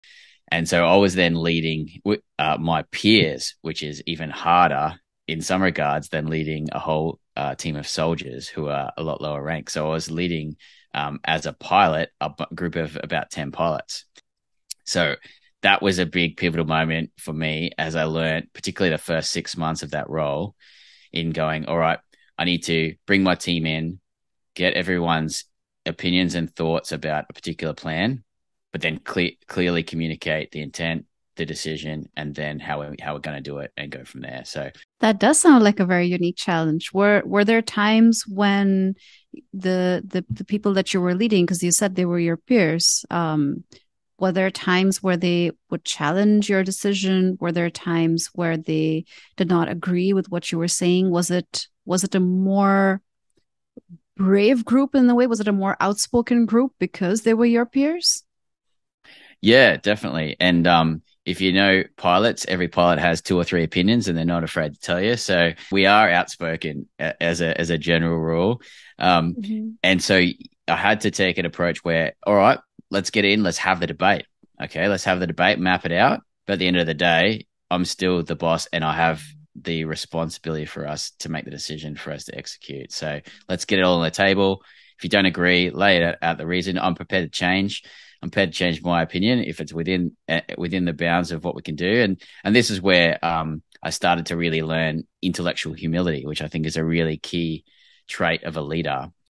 How do you lead people who don't want to be led?
Jamie Martin trains the leaders of tomorrow at the Center for Australian Army Leadership and he shares some truly fascinating lessons from the battlefield and beyond.

I’m your host, Nausheen I. Chen. I help CEOs become public speaking powerhouses.